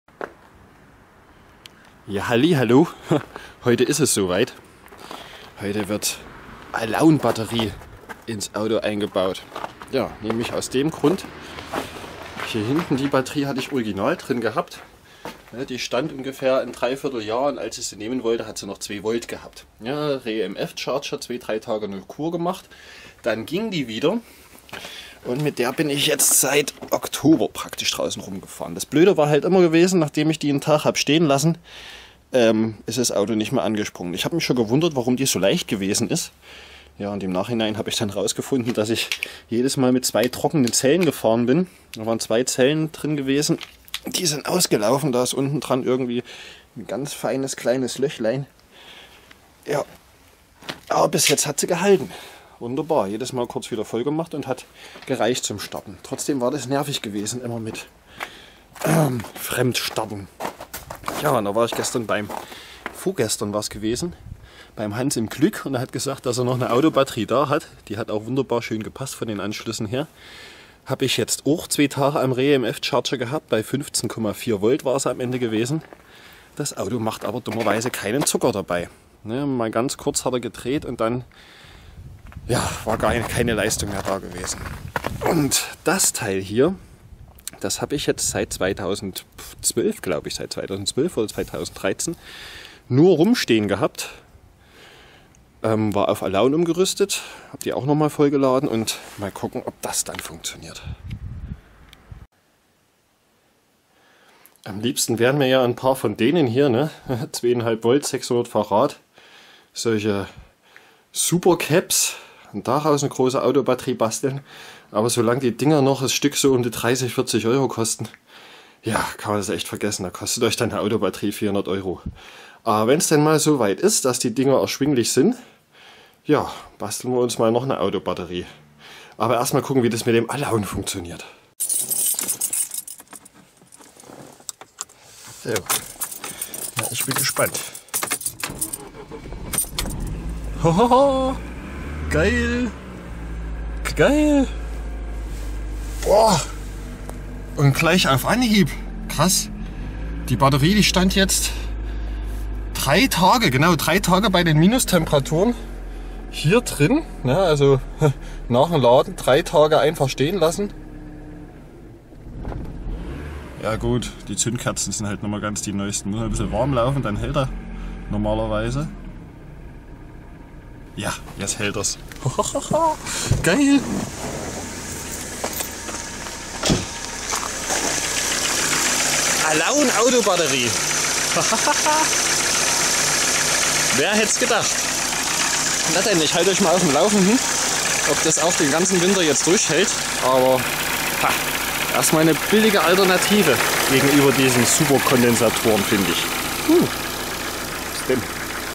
Alaunbatterie startet Auto bei Minustemperaturen
Dies war eines der Dinge mit denen ich am wenigsten rechnete. Eine Autobatterie umgerüstet auf Alaunsalz schafft es den Startstrom für den Anlassermotor zu liefern. Nun wissen wir also auch was passiert wenn man gut funktionierende Autobatterien auf Alaun umrüstet.

Wie Immer Viel Spaß beim ansehen! Denkt dran die Partnerkanäle zu abonieren!